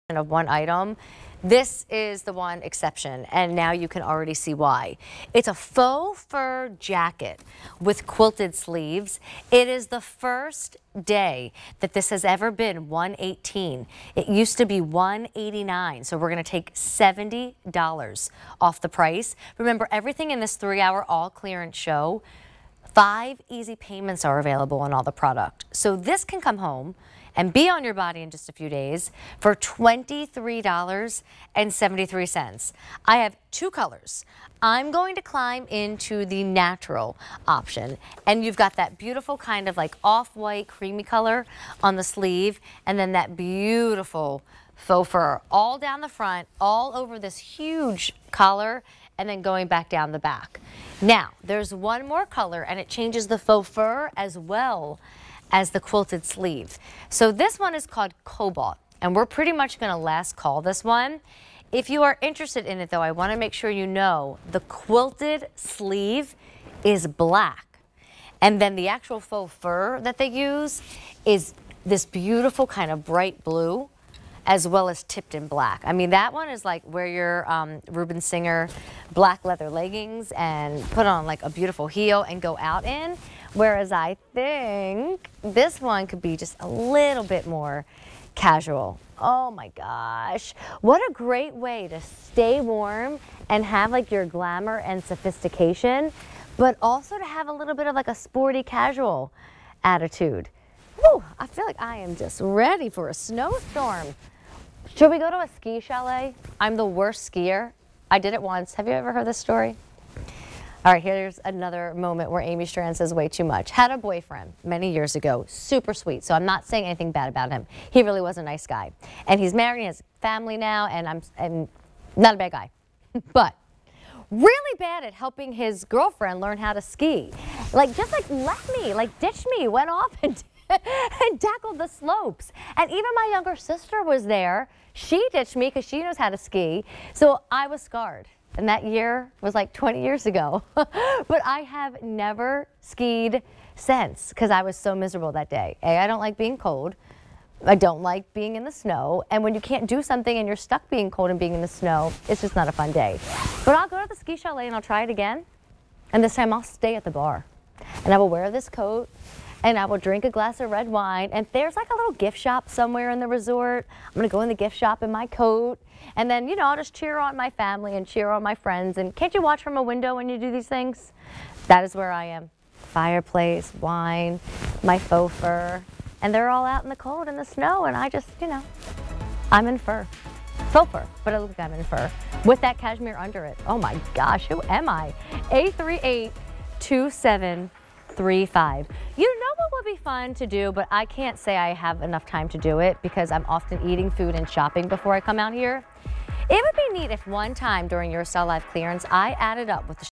Nuage Faux Fur Jacket with Quilted Sleeves on QVC
Nuage Faux Fur Jacket with Quilted Sleeves

Fabrication: woven. Features: faux fur jacket, quilted puffer sleeve, shawl collar, center front hook & eye closure, pckts at waist.  Semi-Fitted - Follows the lines of the body with added wearing ease. Length: Missy Range:28&#34;-30 1/8&#34;. Plus Range:30 1/2&#34;-32 1/4&#34;.________  Content:Shell Faux Fur:45% Modacrylic/41% Acrylic/14% Polyester. Sleeves/Filling/Lining:100% Polyester. Care: Dry Clean. Imported. (F).